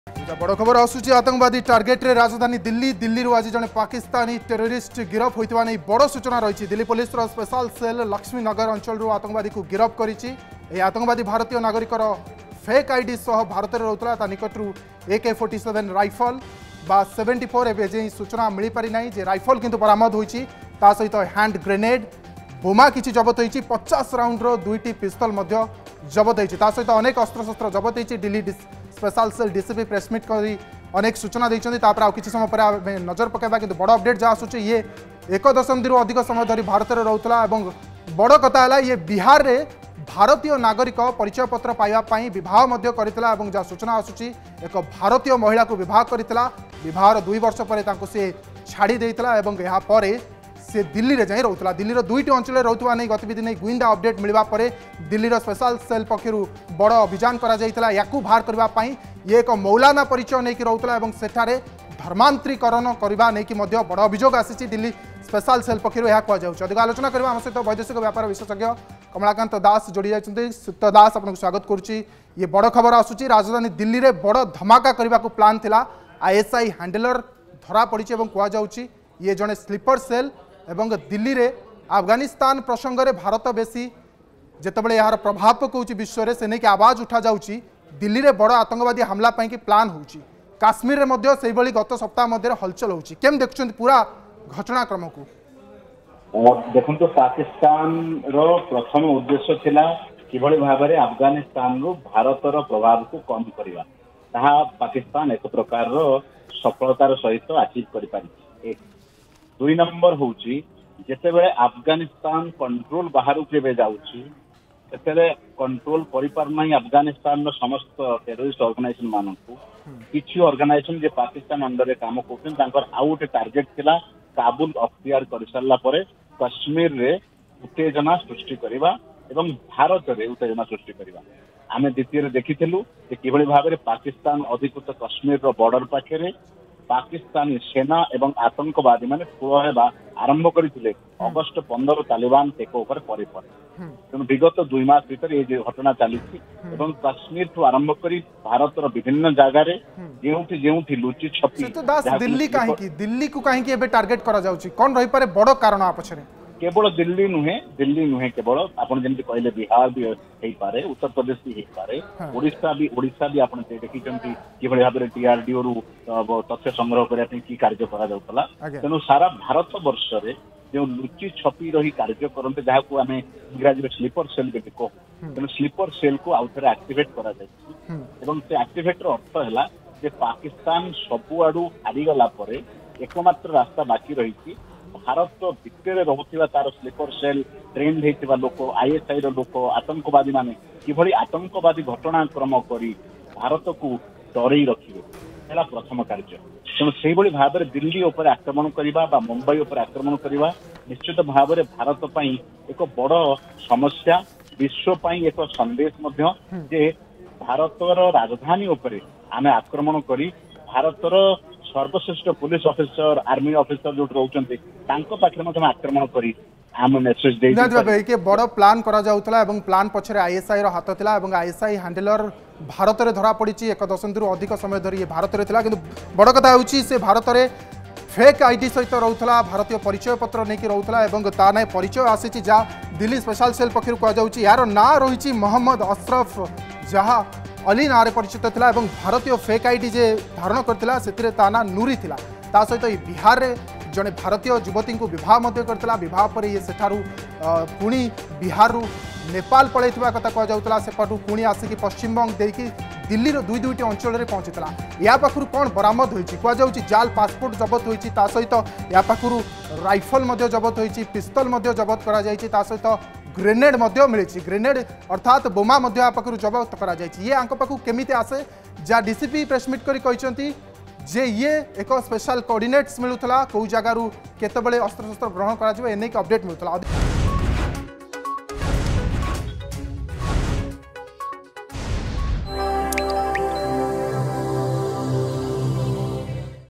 ଦିଲ୍ଳୀରୁ ଆଜି ଜଣେ ପାକିସ୍ତାନୀ ଆତଙ୍କବାଦୀ ଗିରଫ
ଦିଲ୍ଳୀରୁ ଆଜି ଜଣେ ପାକିସ୍ତାନୀ ଆତଙ୍କବାଦୀ ଗିରଫ । ଦିଲ୍ଲୀ ପୋଲିସର ସ୍ପେଶାଲ ସେଲ୍ ଲକ୍ଷ୍ମୀନଗର ଅଂଚଳରୁ ଆତଙ୍କବାଦୀକୁ ଗିରଫ କରିଛି । ଏହି ଆତଙ୍କବାଦୀ ଭାରତୀୟ ନାଗରିକର ଫେକ୍ ଆଇଡି ସହ ରହୁଥିଲା